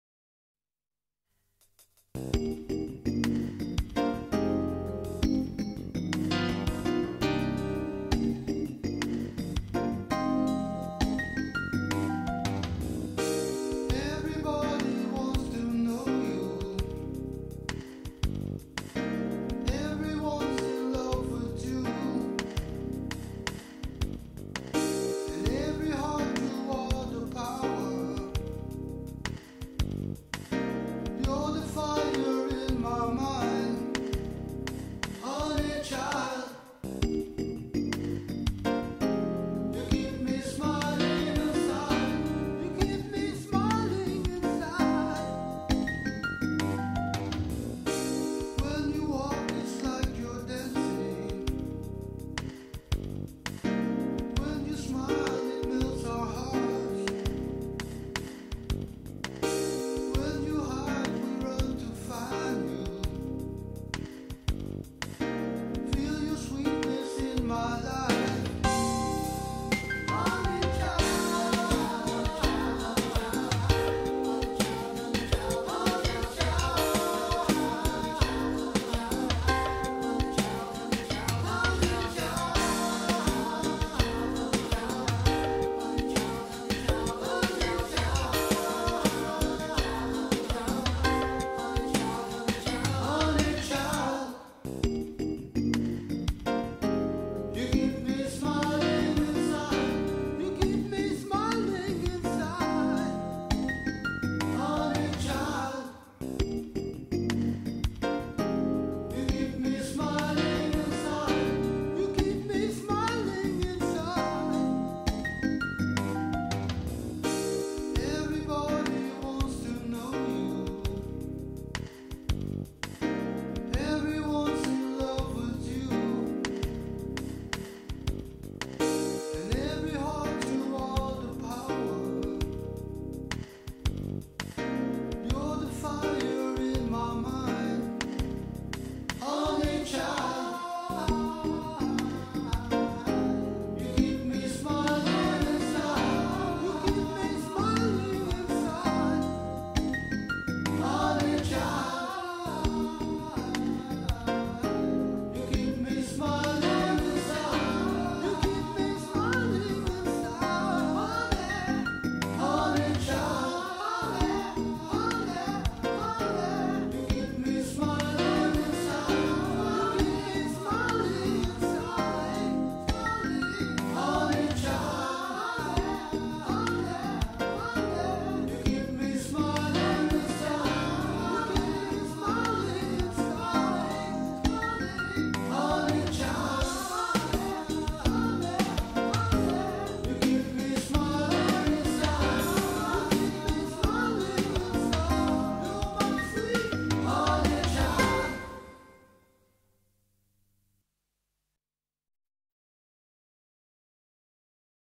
HONEY CHILD
A VIDEO CLIP ABOUT SRI KRSNA IN HIS CHILDHOOD PASTIMES AS SRI DAMODARA